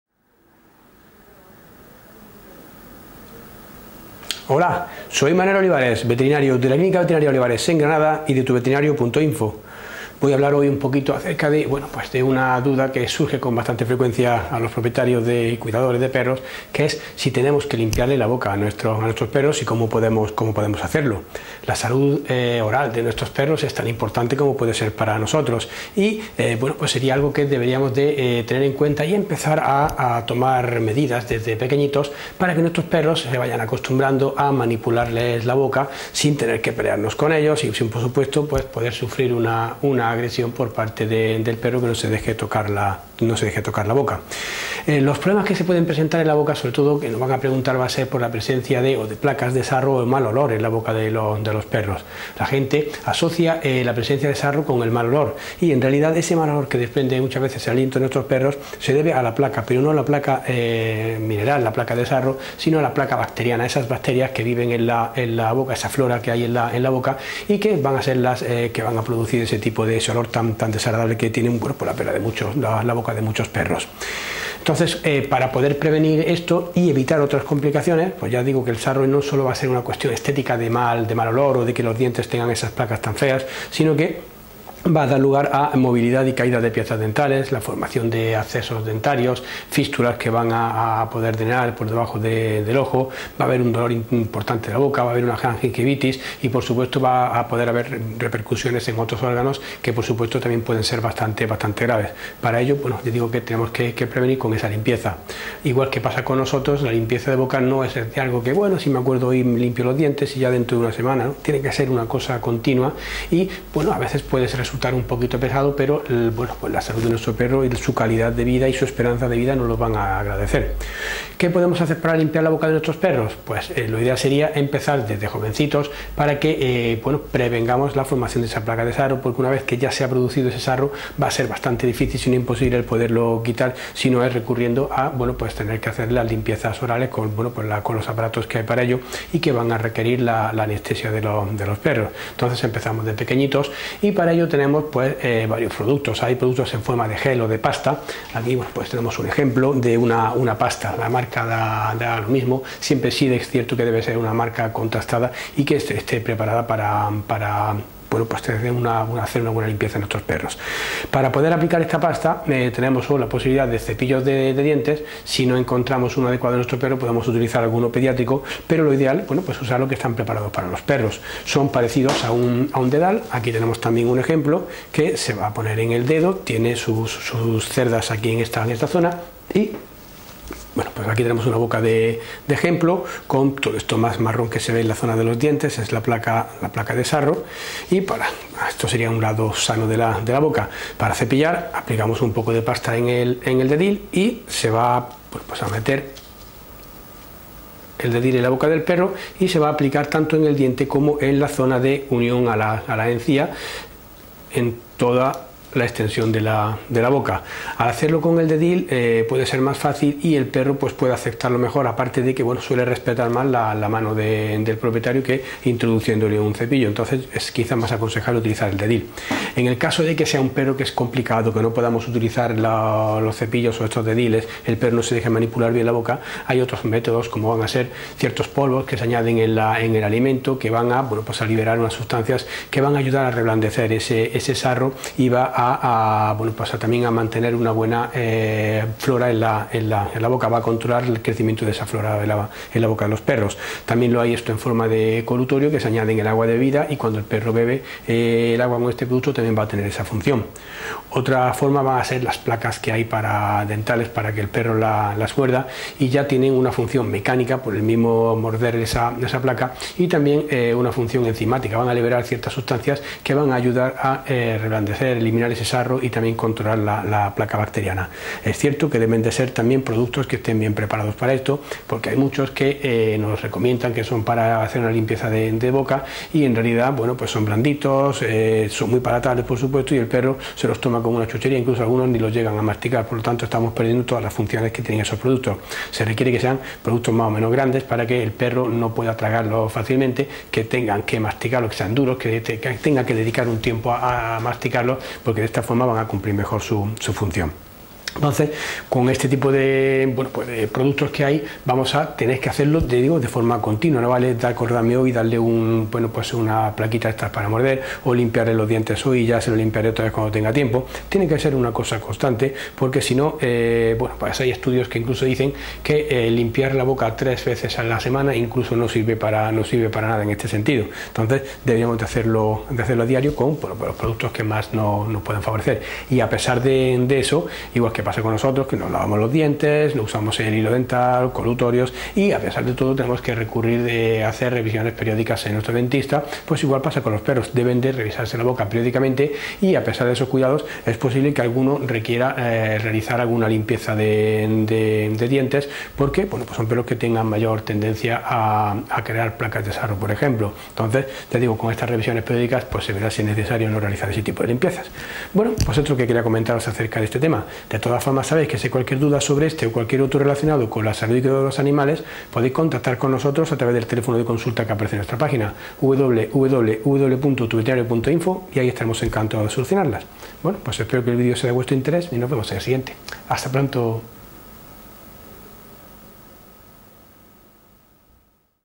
¿Cómo le limpio los dientes a mi perro?
¿Cómo le limpio los dientes a mi perro? Los problemas dentales son muy habituales en nuestros perros y debemos prevenirlos desde el principio.